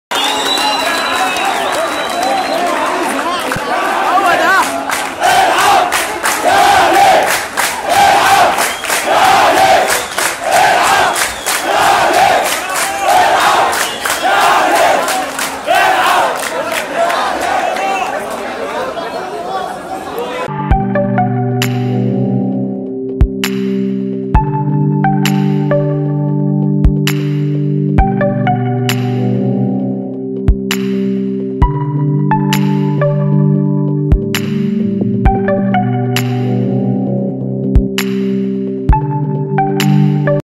فرحة جمهور الأهلي بعد طرد دونجا لاعب بيراميدز
فرحة جمهور الاهلي في مباراة الاهلي وبيراميدز بعد طرد دونجا لاعب بيراميدز

يواجه الأهلي نظيره بيراميدز في مباراة الجولة 27 في الدوري الممتاز بثلاثة لاعبين في خط الوسط، حيث يبحث بيتسو موسيماني مدرب الفريق عن السيطرة على الوسط الدفاعي والهجومي.

تقرير : أحمد موسي

جريدة الوطن
جريدة يومية مستقلة